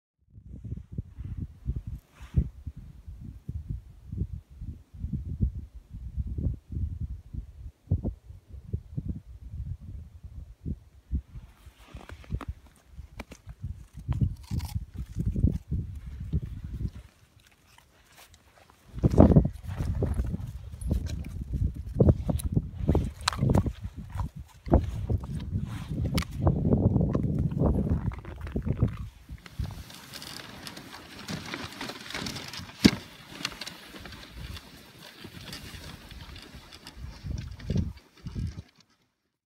Snacks For Sled Dogs On Run/Mushing
Keeping it light and fun for the team with snack breaks in between runs.